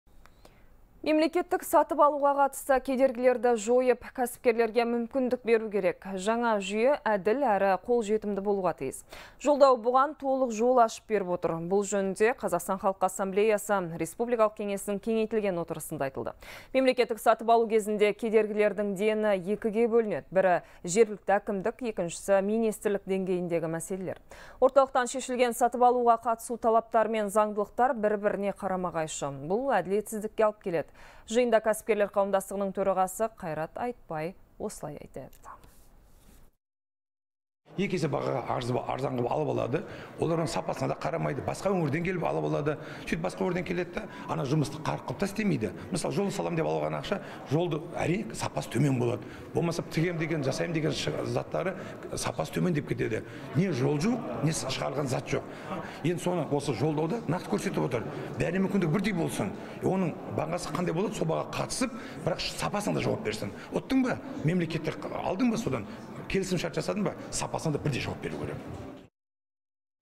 Елде мемлекеттік сатып алу қолжетімді болуы тиіс
Мемлекеттік сатып алуға қатысты кедергілерді жойып, кәсіпкерлерге мүмкіндік беру керек. Жаңа жүйе әділ әрі қолжетімді болуға тиіс. Жолдау бұған толық жол ашып беріп отыр. Бұл жөнінде Қазақстан халқы Ассамблеясы республикалық кеңесінің кеңейтілген отырысында айтылды, деп хабарлайды «Хабар 24».